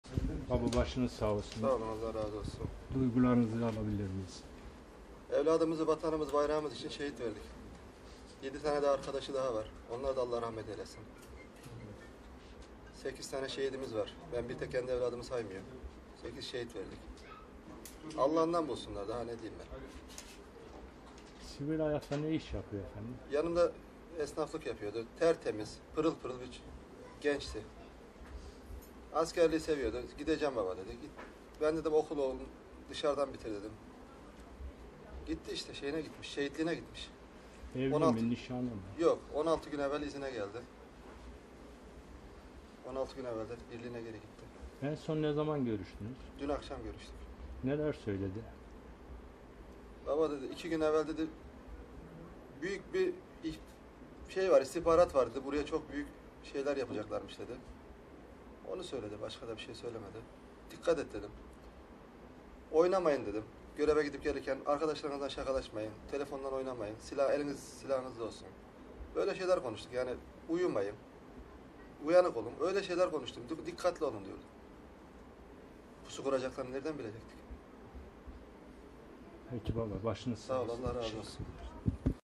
Şehit er Ömer Erüstün'ün acılı babası konuştu
Siirt'in Şirvan ile Pervari ilçeleri arasında PKK'lı teröristlerin karayoluna tuzakladıkları bombanın askeri aracın geçişi sırasında patlatılması sonucu şehit olan 8 askerden biri olan 6 aylık er Ömer Erüstün'ün (20) babası Ali Erüstün, oğluyla en son dün telefonda konuştuklarını anlattı.